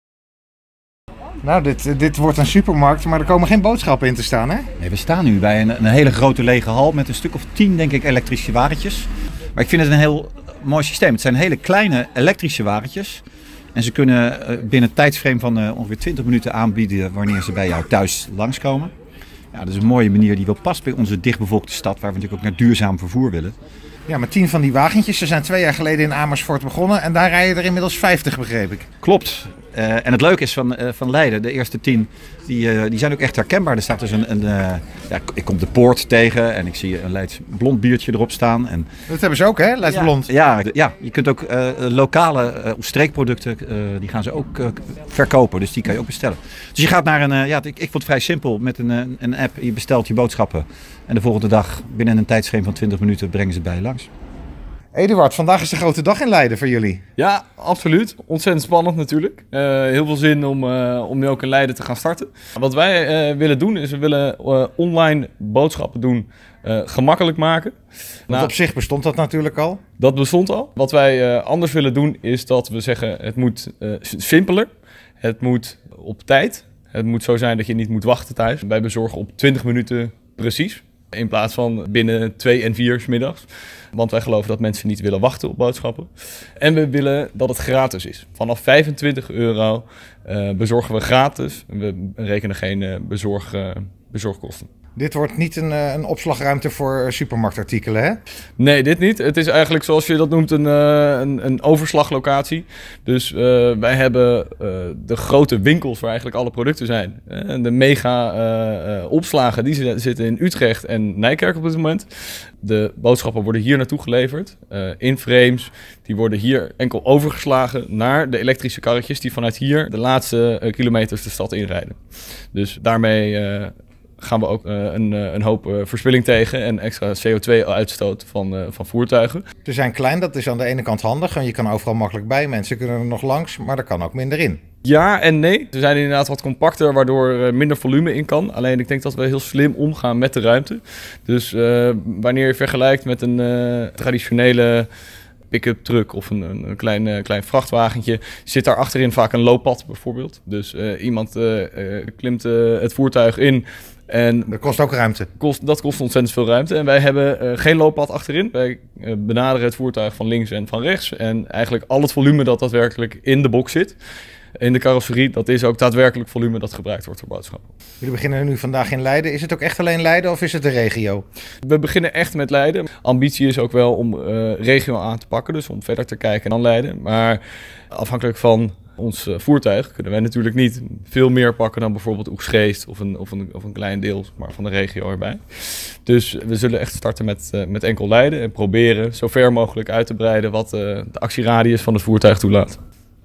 Online supermarkt Picnic gestart in Leiden
Op 25 oktober 2017 is online supermarkt Picnic gestart in Leiden. Aan het woord komen de Leidse wethouder Strijk en Eduard Posthumus Meyjes van Picnic.